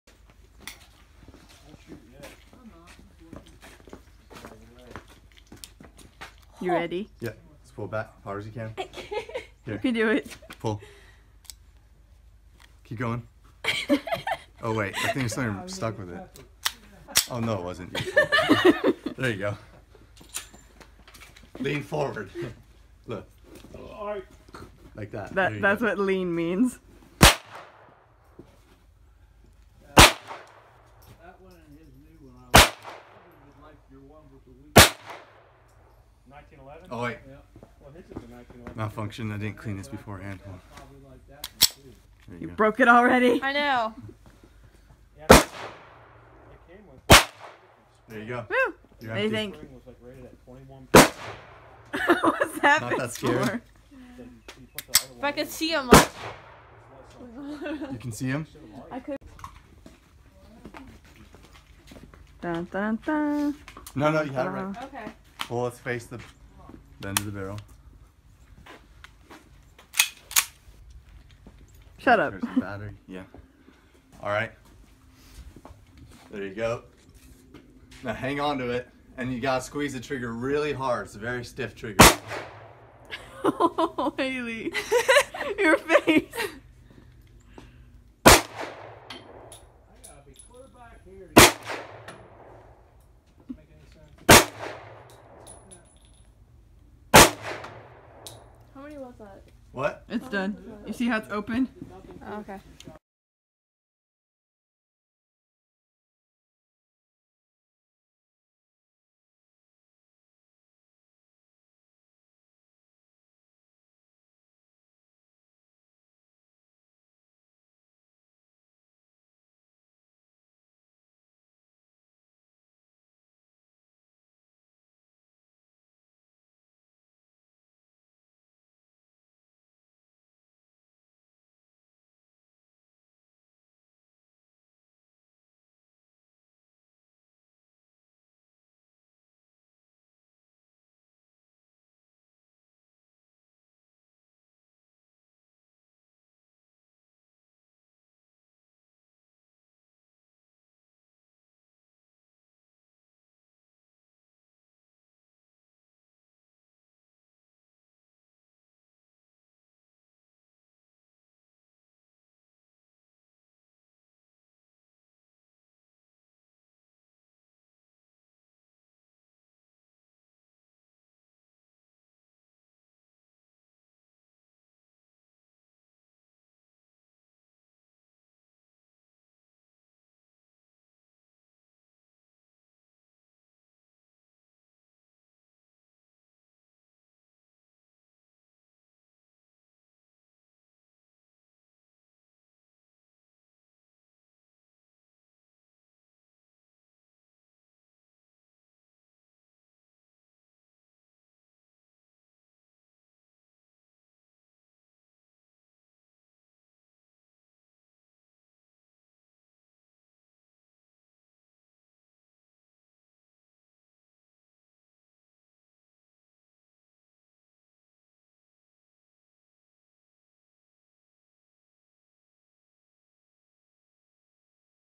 First Time Shooter!
Our friend goes shooting for the first time!